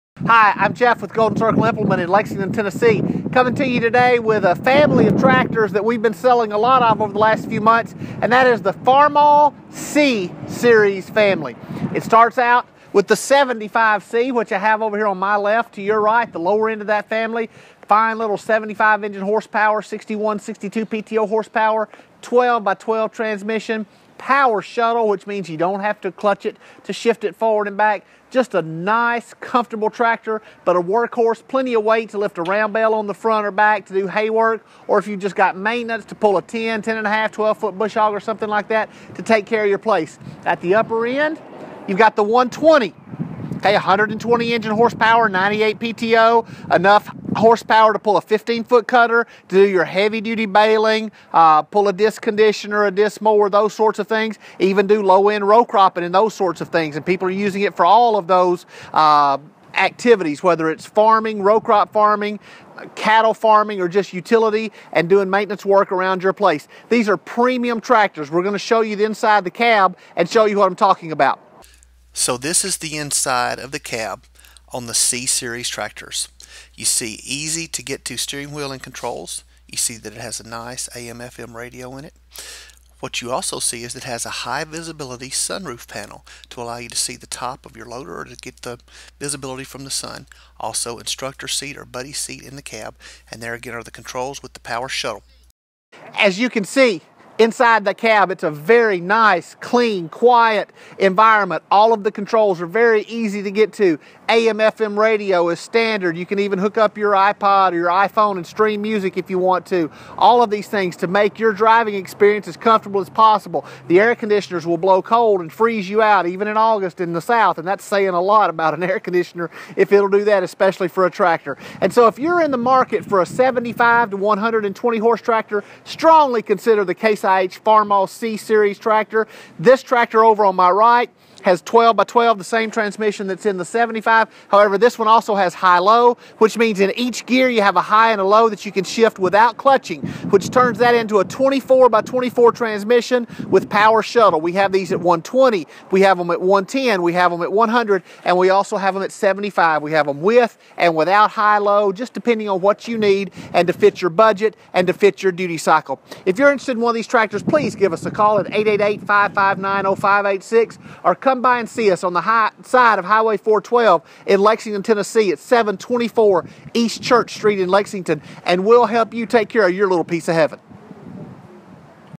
The Full line of Case IH Farmal C Series Tractors
Here is a video on the full line of Farmal C series tractors.

Or visit us at 724 East Church ST Lexington 38351